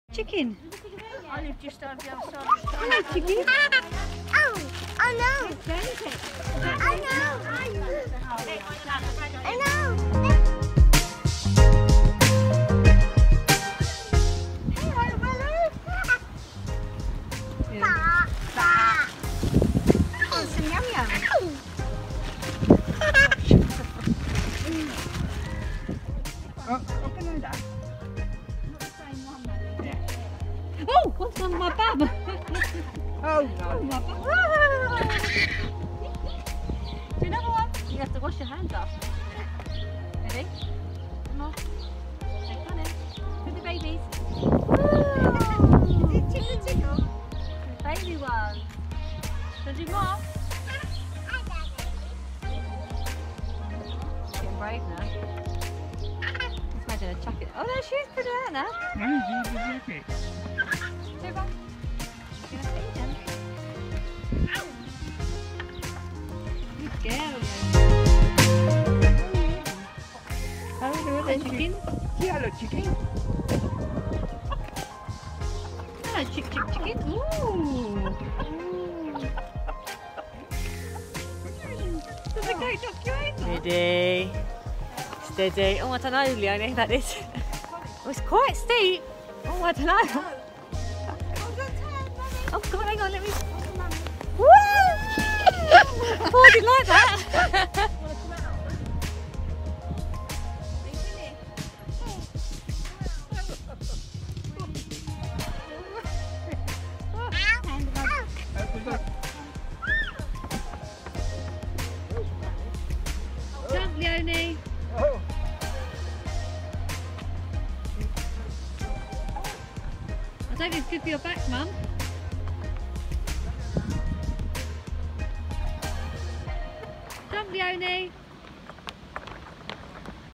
Brooks Farm Leyton
An family afternoon visiting Brooks Farm In Leyton. This was Leonie’s first time feeding the goats 🐐
Devon managed to sleep through the whole outing! 😂